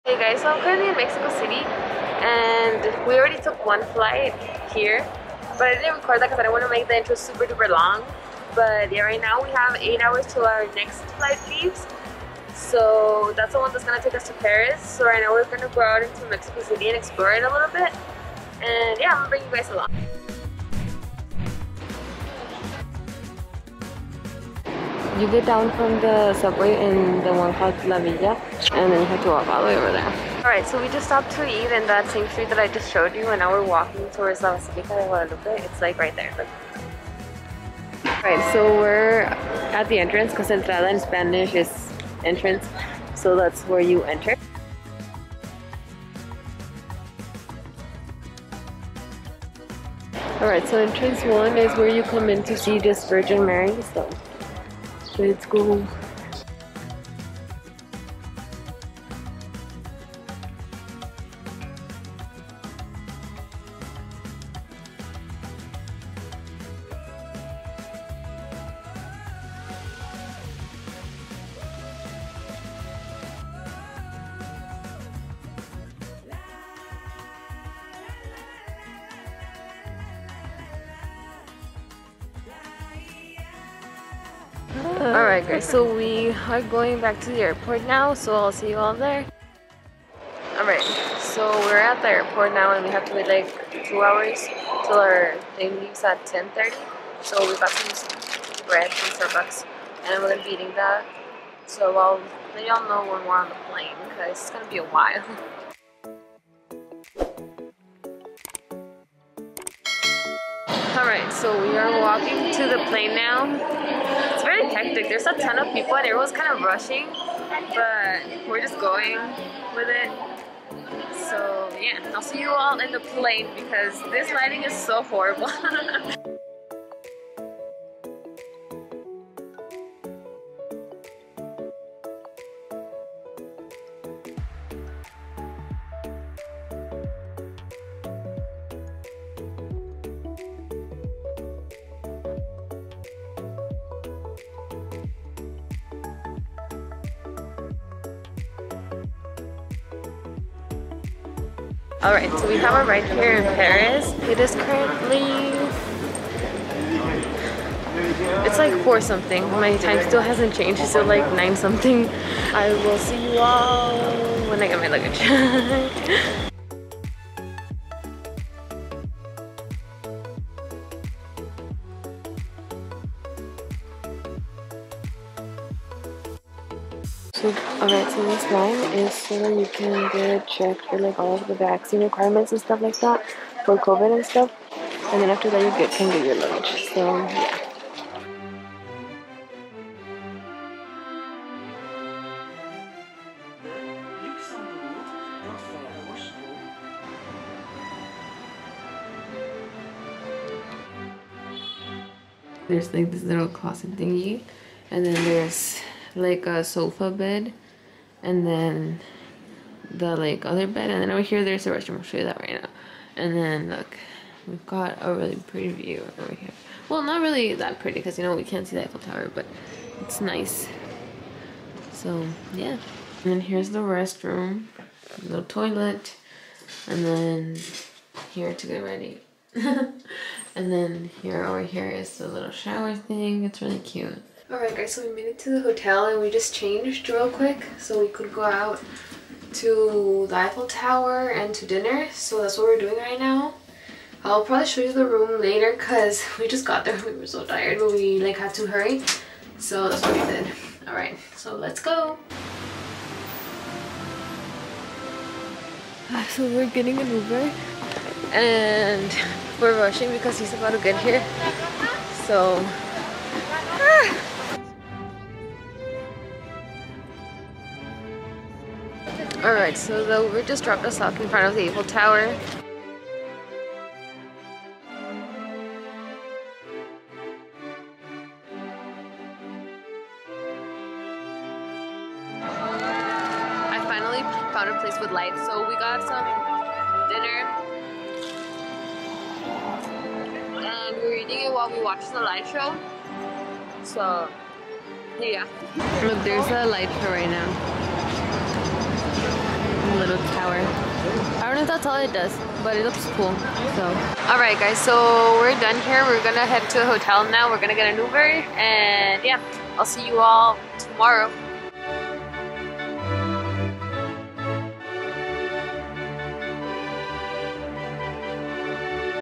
Traveling to Paris, France 2021: Stopping at the Basilica de Guadalupe + Arriving at Paris | Leslie
For the longest time a dream of mine had been to visit Paris, France. I honestly can't believe I actually got to experience this. It is surreal to say that I went to Paris, especially during a world pandemic. I am honestly so thankful with life for allowing me to experience such amazing things. Over the coming weeks I will be posting the rest of the trip. A little spoiler: I didn't just go to Paris, France.

Get Ready With Me for a Wedding (I didn't know what I was doing half of the time)